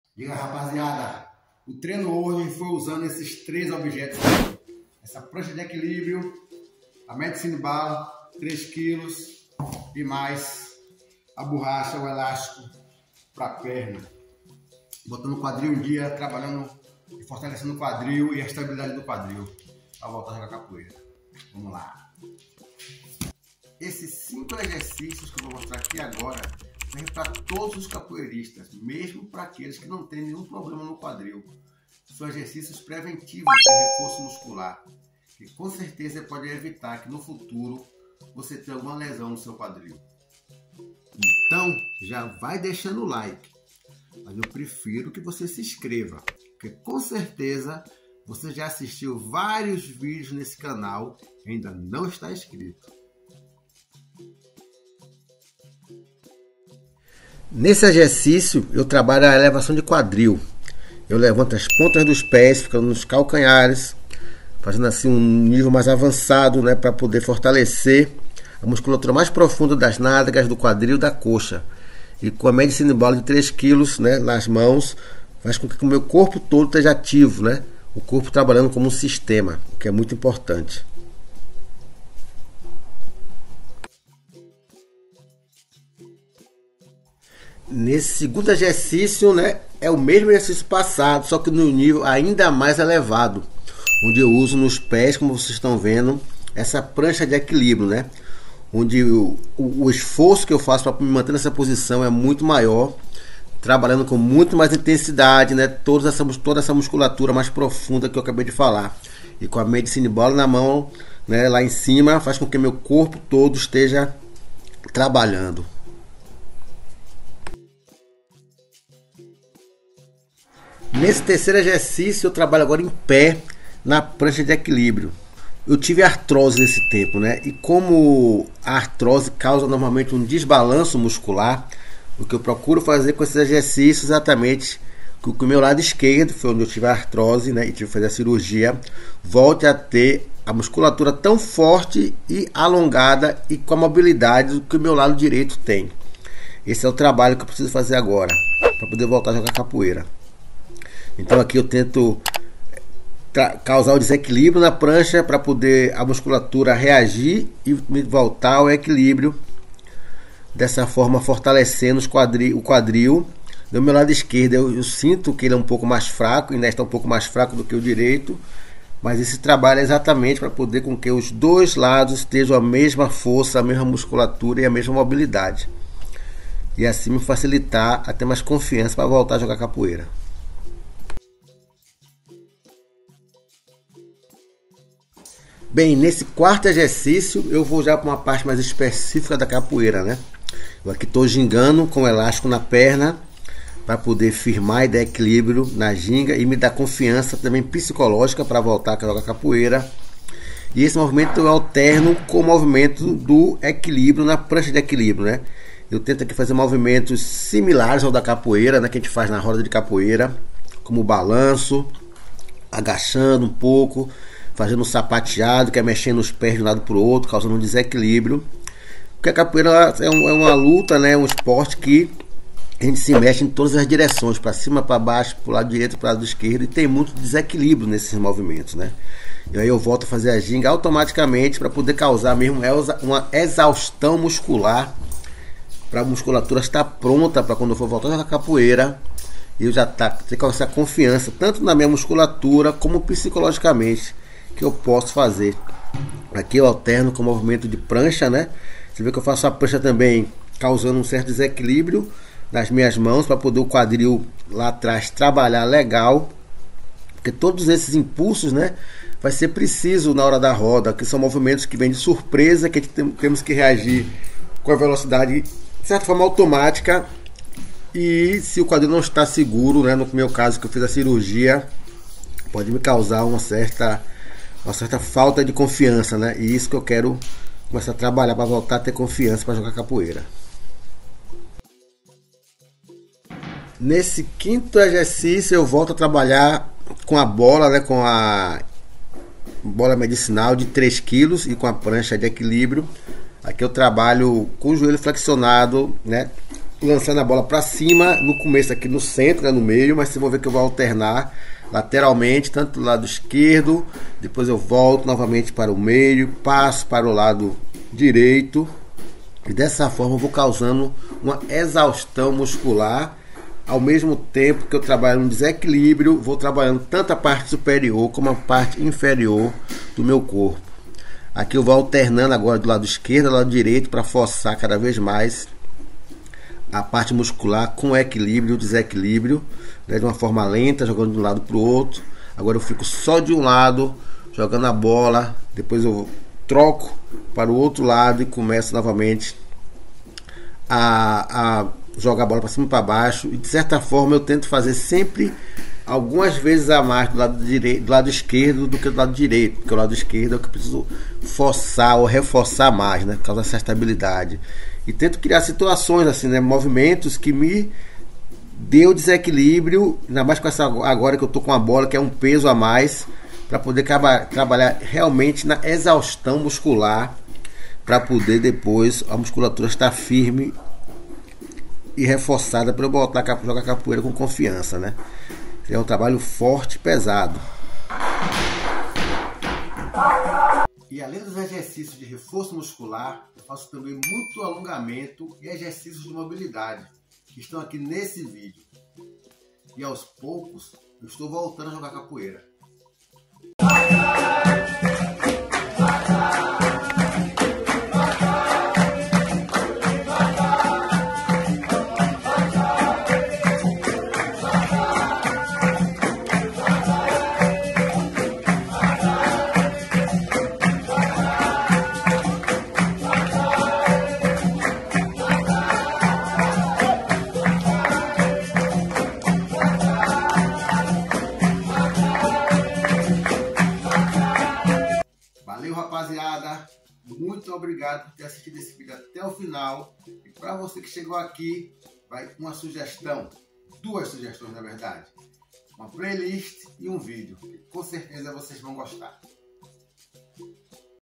🟢 Voltando a jogar capoeira | TREINO DE FORÇA
Já se passaram mais de 5 anos desde que comecei a ter dor no quadril, mas pensei que era apenas dor muscular.
Jogo capoeira desde os 13 anos, além de surfar e jogar bola como hobby. A capoeira virou profissão.

Neste momento, depois de fazer uma cirurgia no quadril, estou iniciando fisioterapia e reabilitação para voltar primeiro
andar sem muletas e depois comecar aos poucos com exercícios de capoeira e outros esportes que gosto de praticar.

A ideia é postar vídeos com o desenvolvimento da reabilitação até que comece a jogar capoeira, provavelmente com alguma limitação.

Videos novos toda semana

♻️Quer apoiar o Canal?♻️ === 😍O VALOR VOCÊ DECIDE😍
Brasil pelo Pix